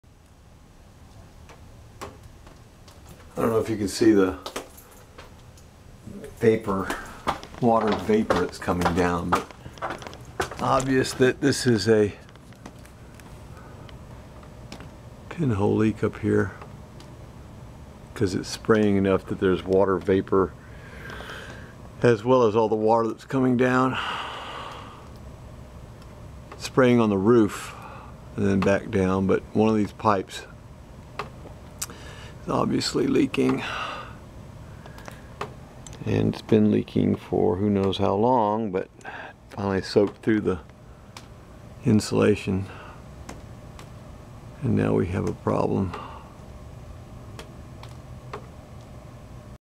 Pinhole Leak @ Work
Figured it would be good to document this water leak at our shop...

You probably need this to be at 720 and full screen to see the water dripping from the red iron...